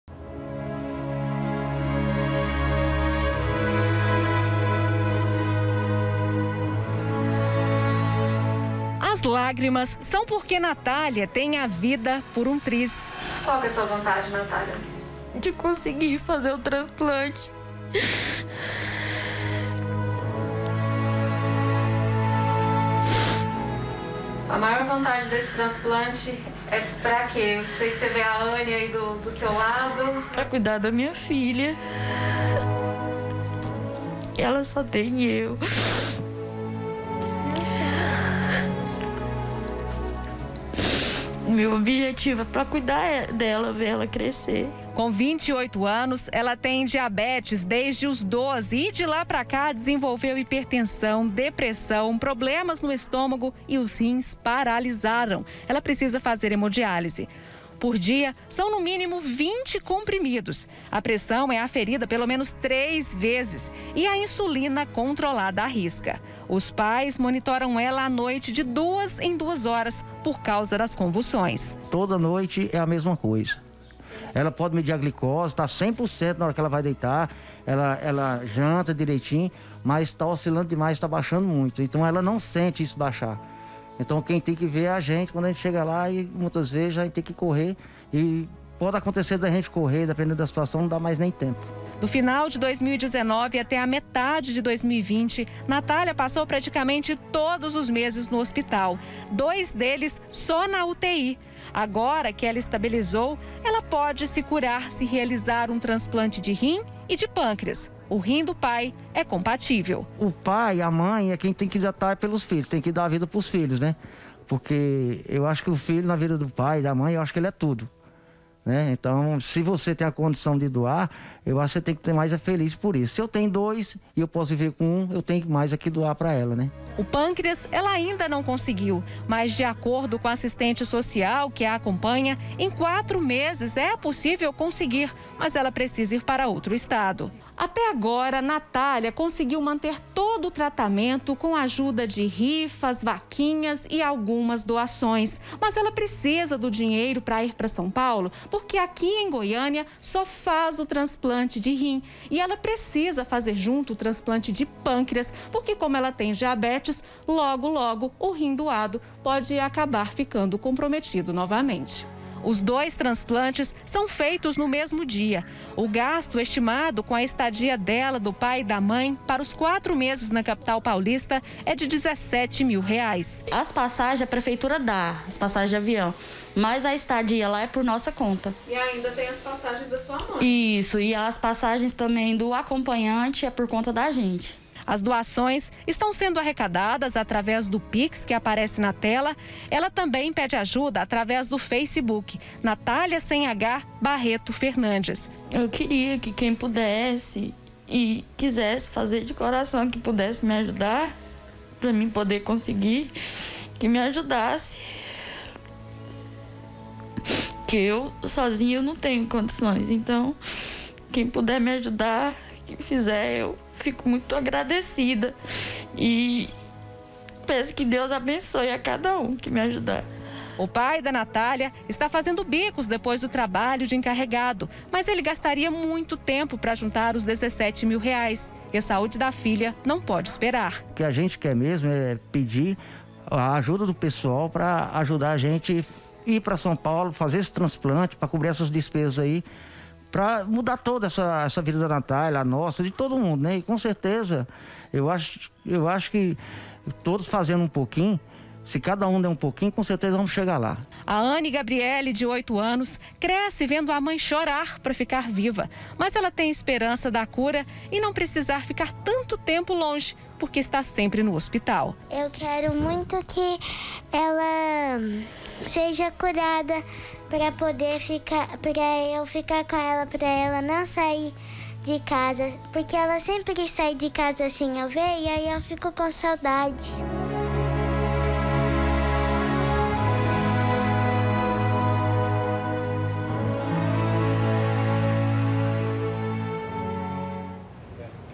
LUTA PELA VIDA: NATÁLIA PRECISA DE AJUDA PRA FAZER TRANSPLANTES
EU VOU TE MOSTRAR AGORA A HISTÓRIA DA NATHÁLIA. ELA SÓ TEM 28 ANOS, MAS JÁ BATALHA PELA VIDA. ELA É DIABÉTICA, HIPERTENSA E AGORA RENAL CRÔNICA. PRA SE CURAR, ELA PRECISA FAZER UM TRANSPLANTE DE RINS E DE PÂNCREAS EM SÃO PAULO.